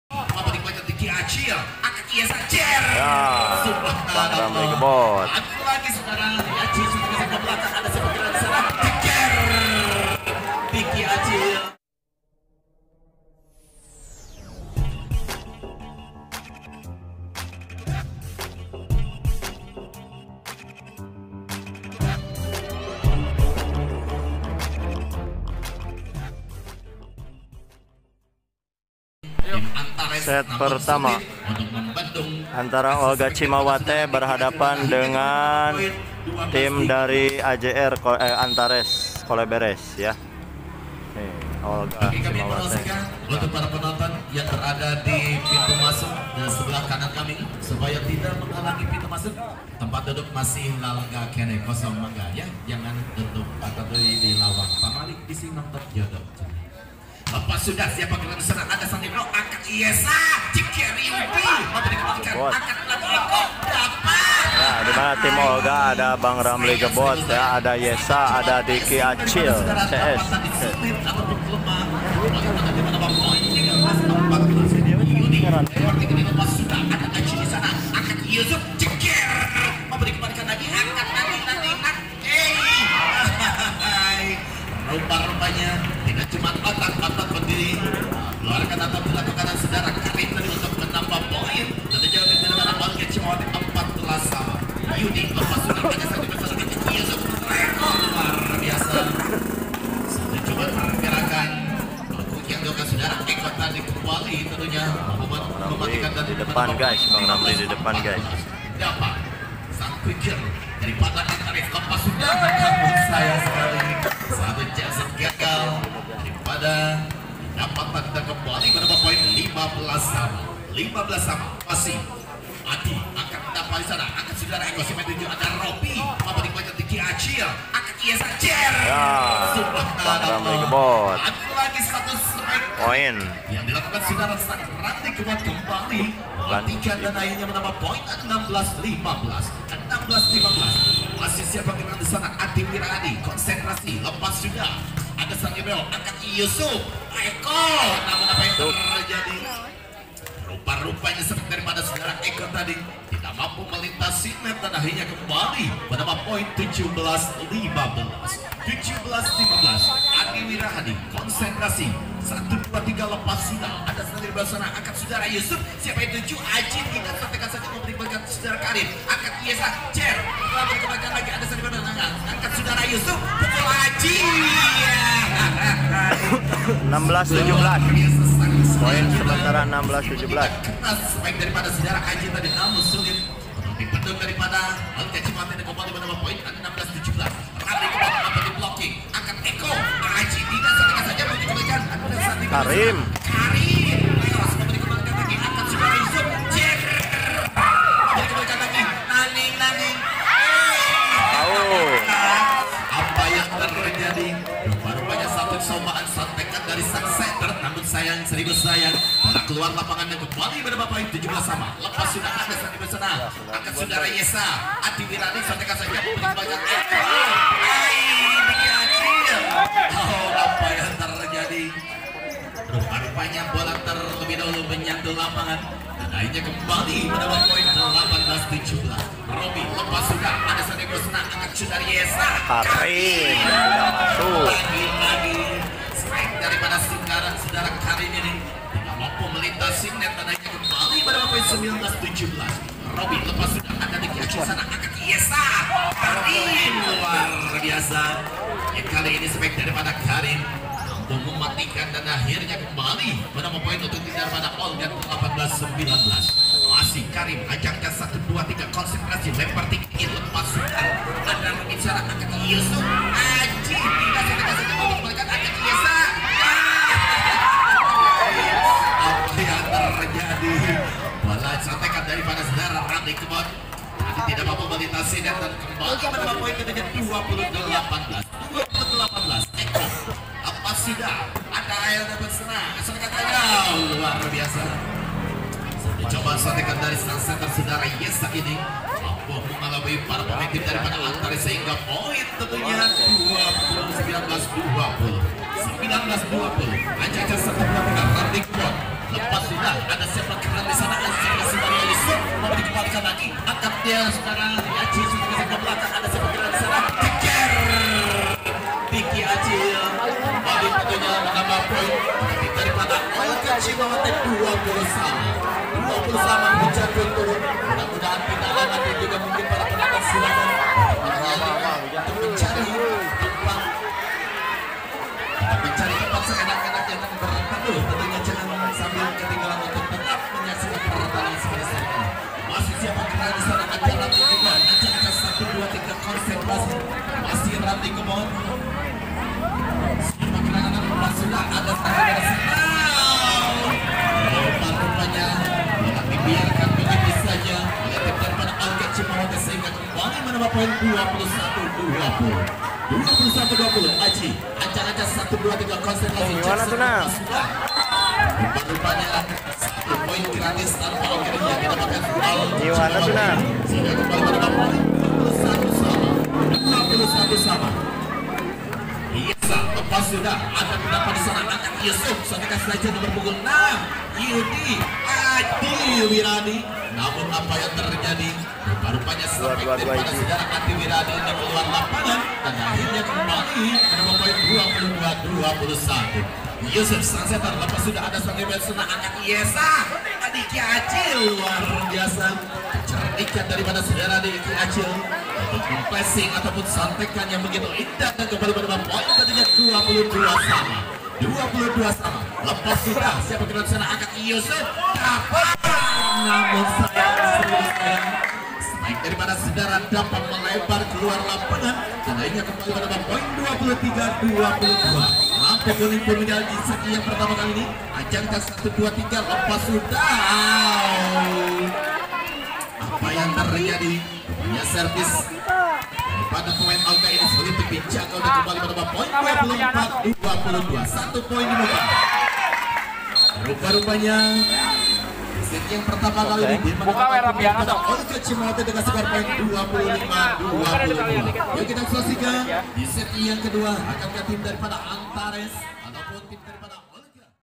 QUIK KHAS LOMPATAN KATAK TANPA AWALAN RAMLI GEBOT HEBOHKAN GBC CUP 2024
GBC CUP 2024
OLGA CIMAWATE VS ANTARES
SET 1
LAPANG VOLI GALUMPIT SUKARAJA TASIKMALAYA JABAR.

SEMOGA TERMOTIVASI !

TERIMAKASIH .
BERLATIHLAH KARENA SEORANG MASTER DULUNYA SEORANG AMATIRAN !!!